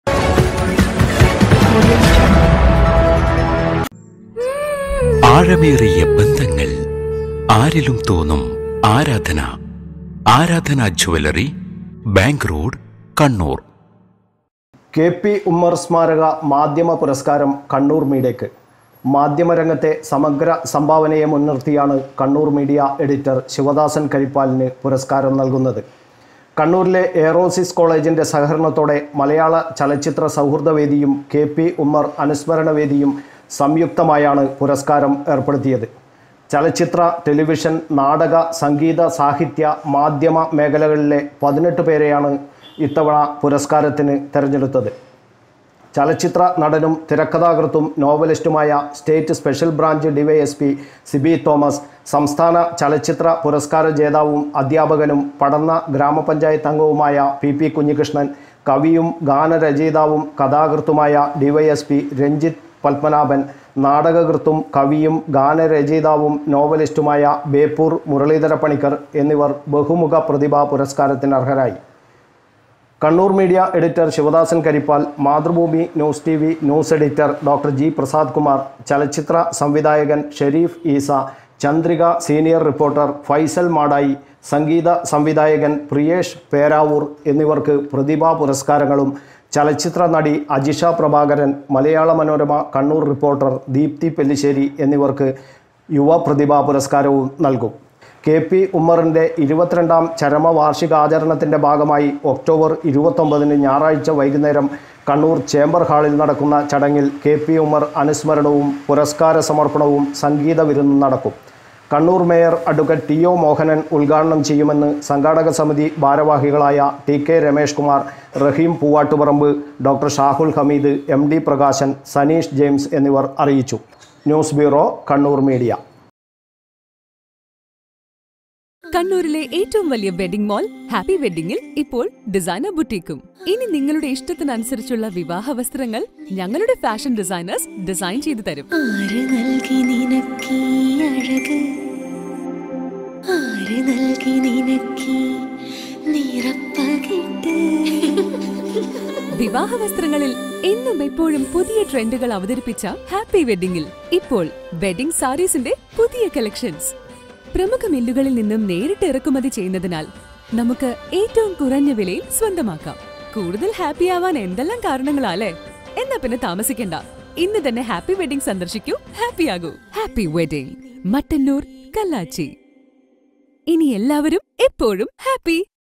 കെ പി ഉമ്മർ സ്മാരക മാധ്യമ പുരസ്കാരം കണ്ണൂർ മീഡിയക്ക്‌.
കെ പി ഉമ്മർ സ്മാരക മാധ്യമ പുരസ്കാരം കണ്ണൂർ മീഡിയക്ക്‌. മാധ്യമ രംഗത്തെ സമഗ്ര സംഭാവന മുൻനിർത്തിയാണ് കണ്ണൂർ മീഡിയ എഡിറ്റർ ശിവദാസൻ കരിപ്പാലിന് പുരസ്കാരം നൽകുന്നത്